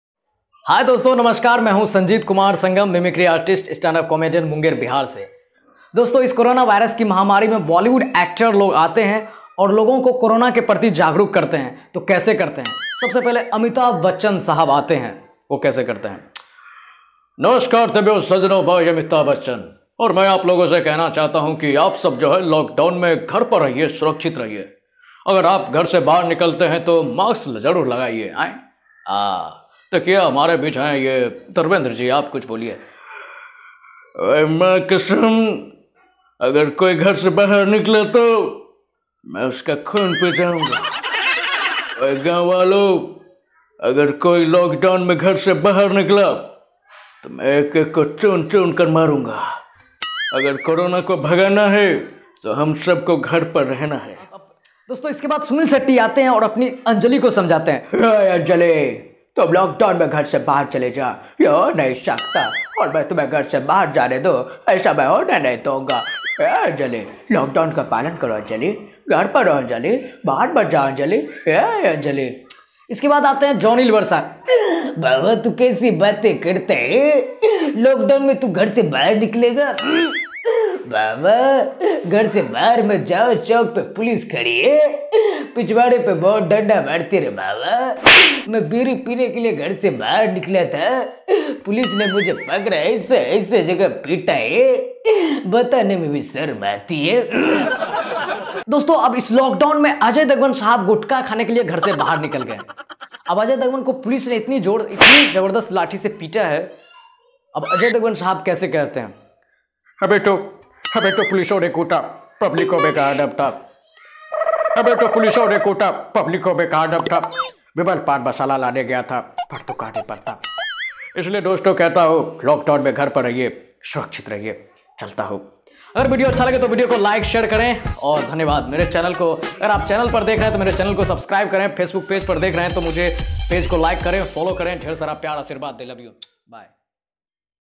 बिहार इस लड़के ने इन सभी कलाकारों का हूबहू आवाज निकाला आप सब जरूर देखें Best mimicry by sanjeet
Hii

इस बिहारी ने सभी इन सभी कलाकारों का हूबहू आवाज निकाल  निकाल कर कोरोना के प्रति लोगों को जागरूक कर रहे है  जरूर सुने। Best Mimicry by Sanjeet